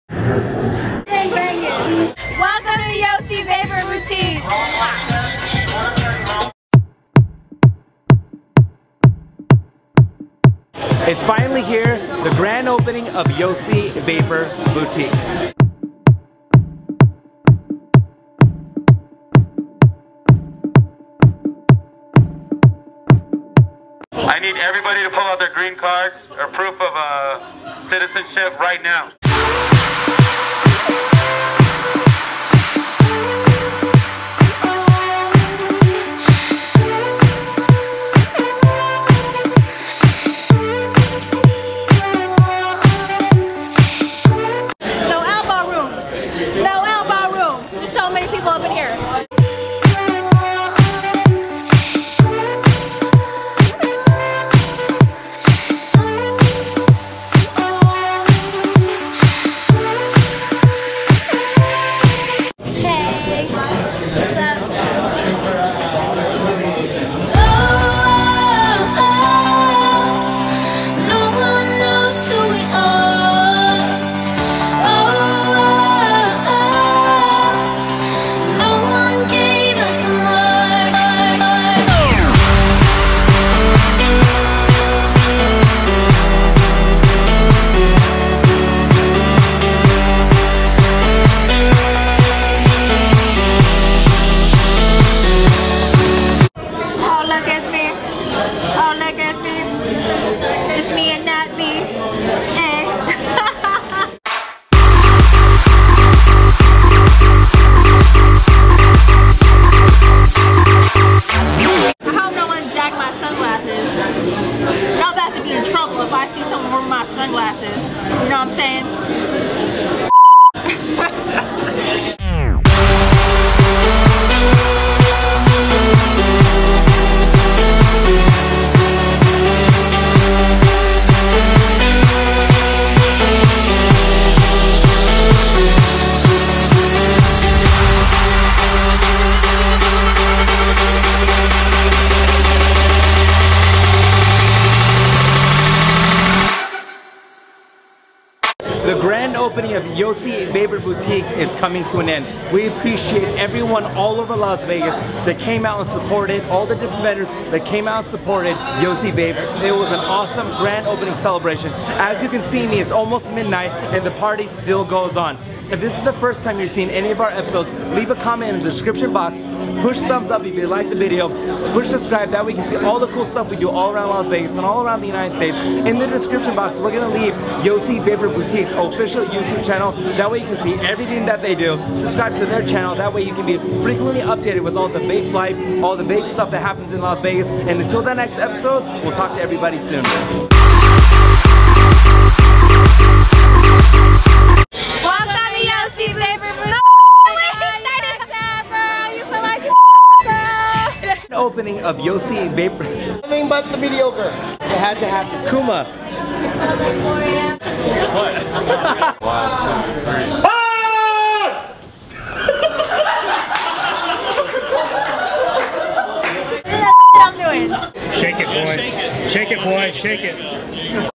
Best Vape Las Vegas; Grand Opening Yosi Vapor Boutique w/ Genevieve Chanelle & Natalie Bee
5115 Spring Mountain Road Suite #119
Las Vegas, Nevada 89146